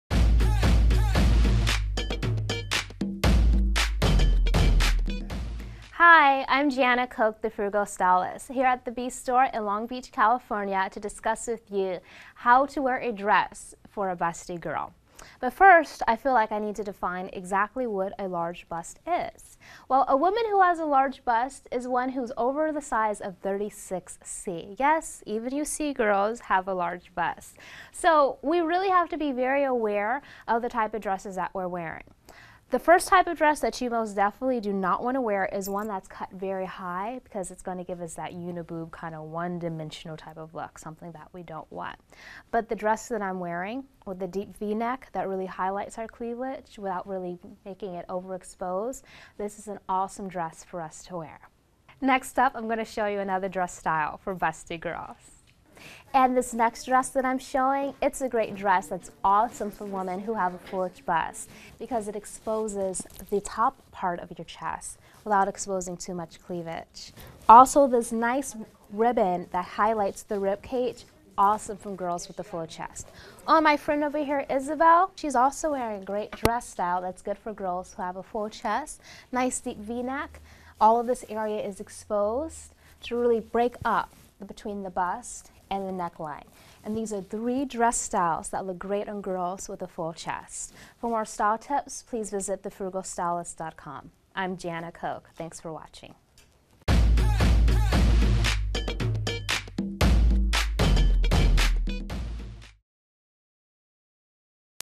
Dress Styles for Busty Girls
Dress Styles for Busty Girls. Part of the series: Fun With Fashion. Certain dress styles look absolutely fabulous for naturally busty girls. Find out about dress styles for busty girls with help from a fashion and style expert in this free video clip.